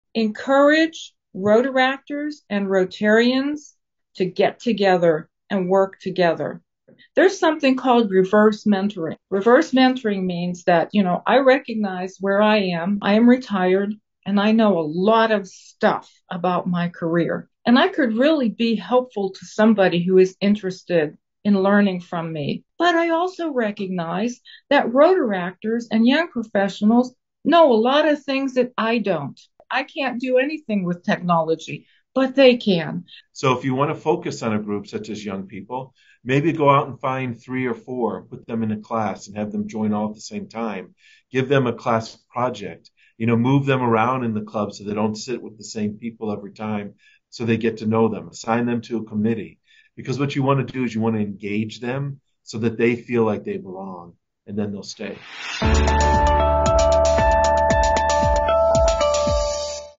S1E09 Youth Driven Rotary Growth
Rotary International Reels on Membership Matters by Rotary International President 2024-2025 Stephanie Urchick and Aide PDG Tom Gump.